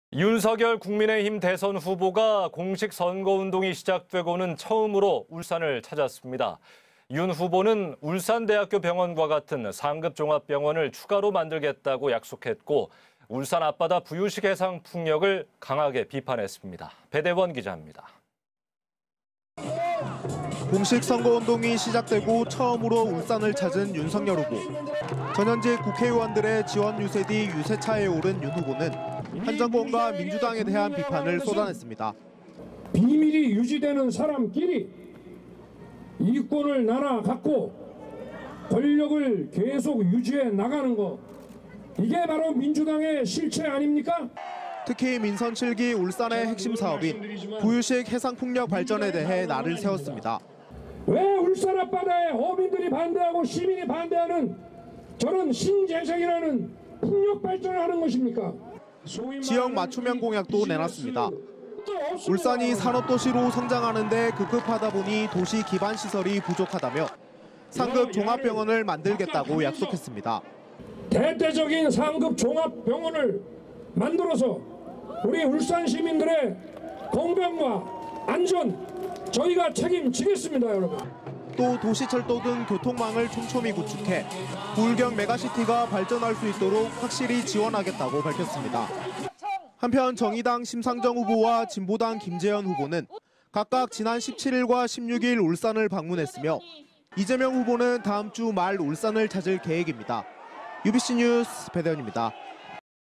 윤석열 상급종합병원 설립 부유식 풍력 반대 220219
윤석열 국민의힘 대선 후보가
공식 선거운동이 시작되고
처음으로 울산을 찾았습니다.

 윤 후보는 울산대학교병원과
같은 상급종합병원을
추가로 만들겠다고 약속했고
울산 앞바다 부유식
해상풍력을 강하게 비판했습니다